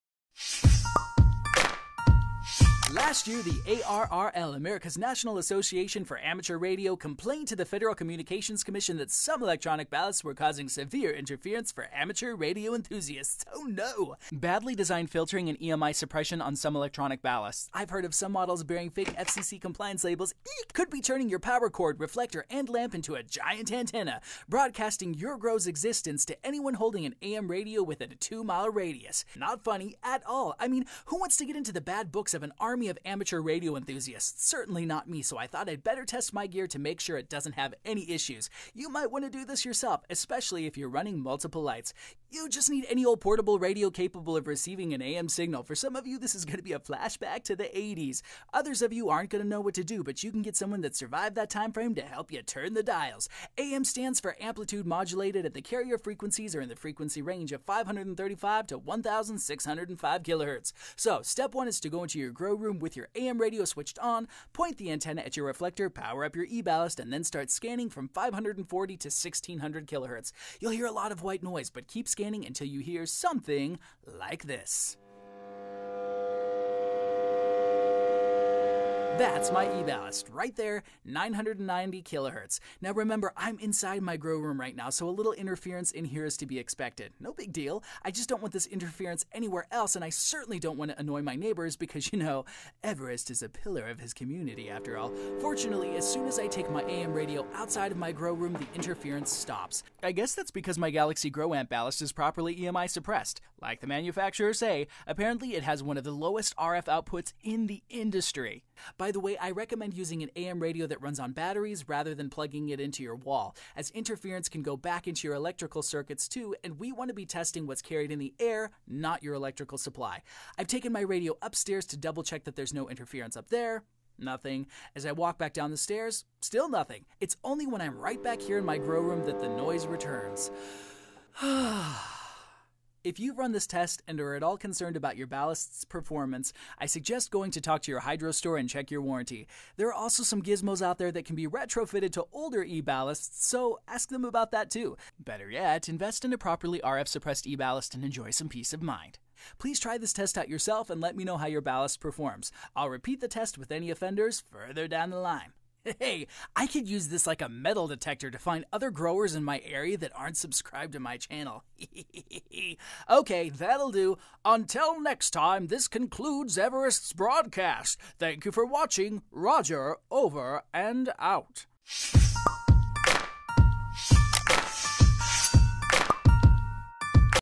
Is Your Grow Room Annoying Your Neighbors?
Some electronic ballast are not properly EMI suppressed and are broadcasting EMI / RF Interference on AM Radio bands and beyond. Is your e-ballast FCC compliant or are you broadcasting your grow's existence to your neighborhood? We try to get to the bottom of the matter.